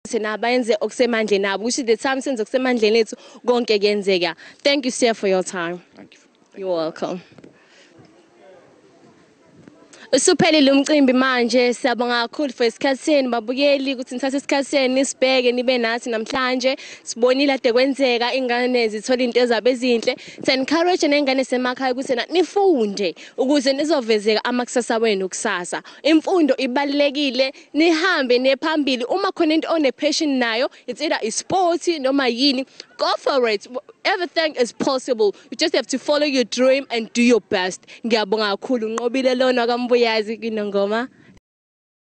THE FIRST EVER INTERVIEW ON THE GROOVE
As we are celebrating 5 years of existence.We will show you what happened during these 5 year period .Here is the presenter of the show On the groove giving motivational talk after finishing interview with one of the  school headmasters.